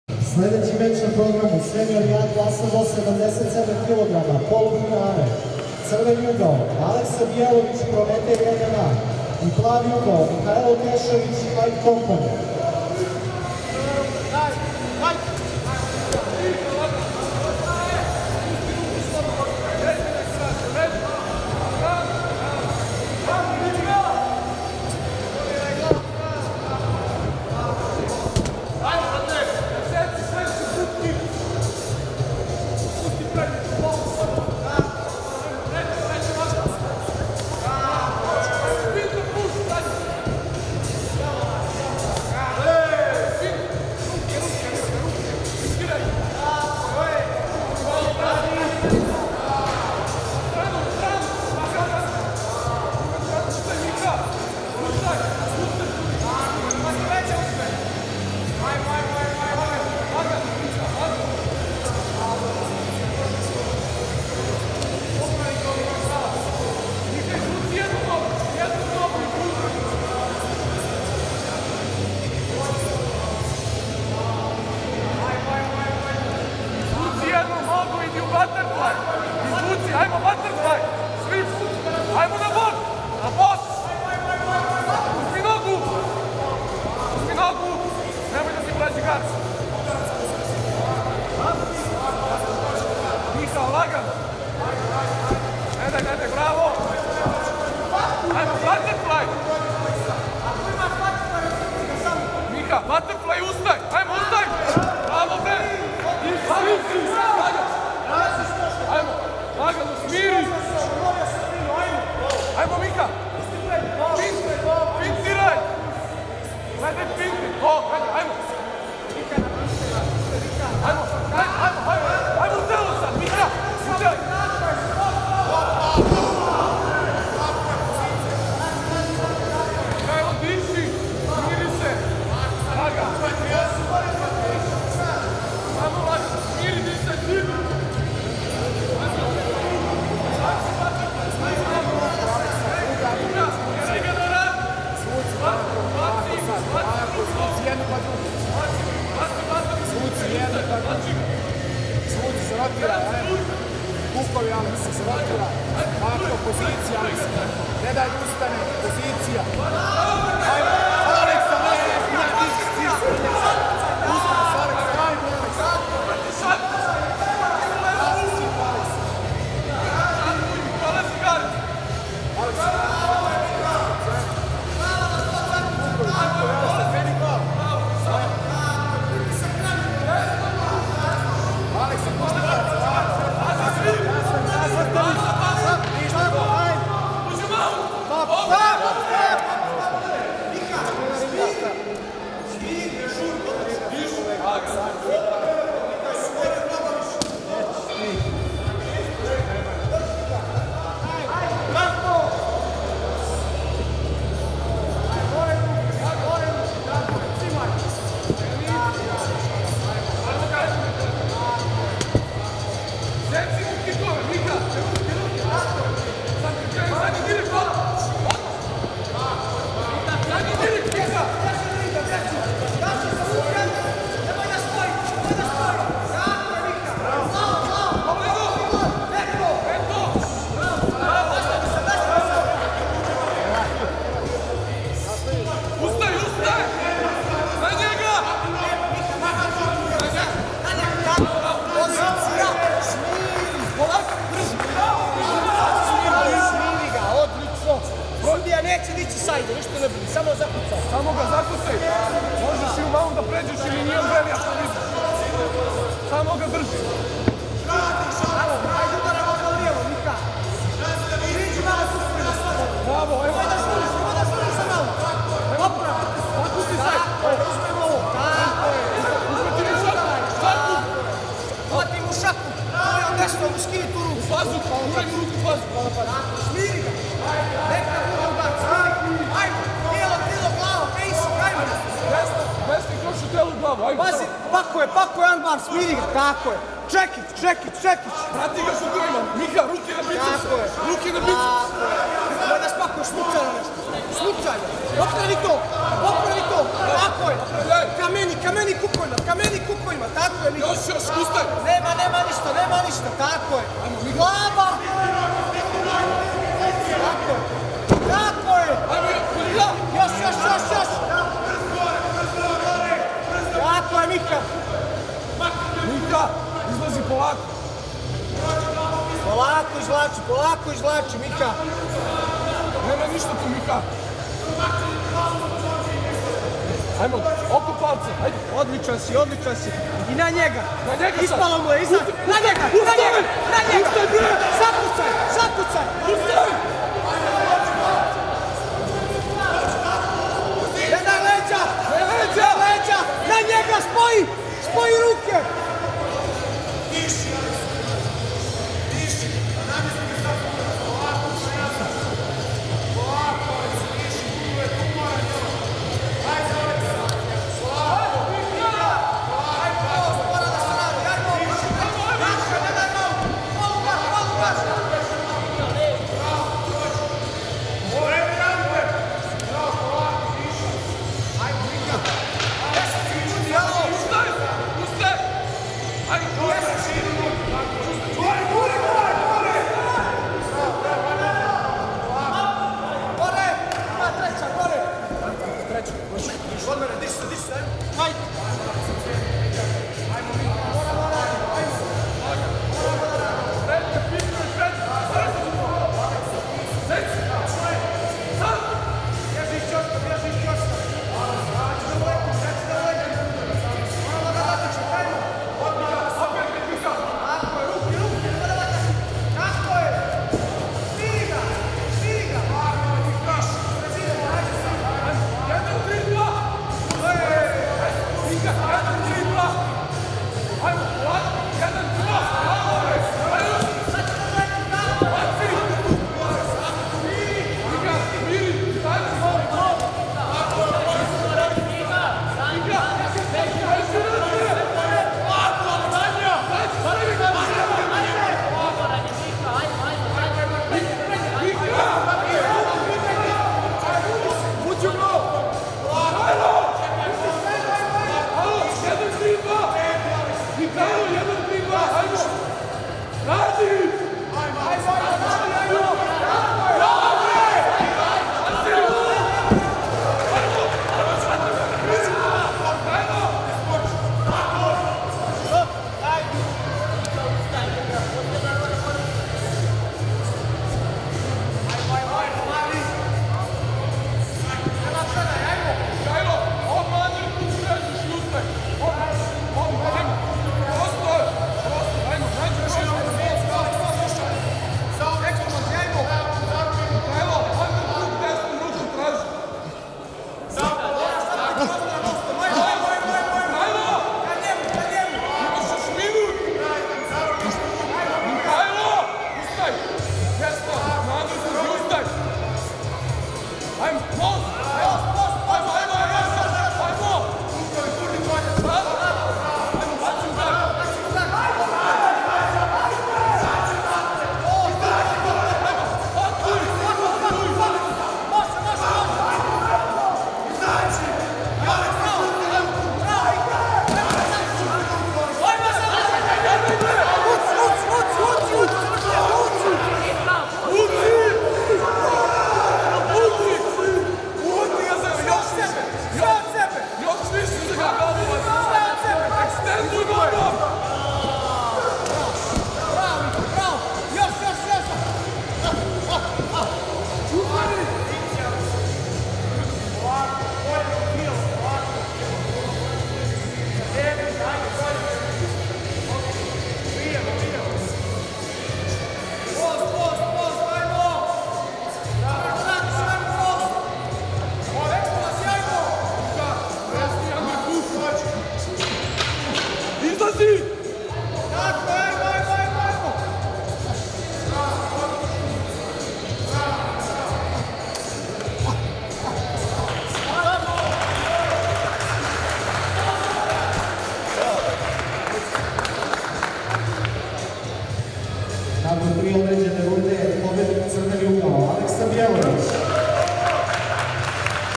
FG13-Aleksa Bijelović (Prometej) vs Mihailo Tešović (Fight Company)
Meč br.50 Seniori A klasa -77kg 1/2 finala
Pobednik: Aleksa Bijelović (Prometej)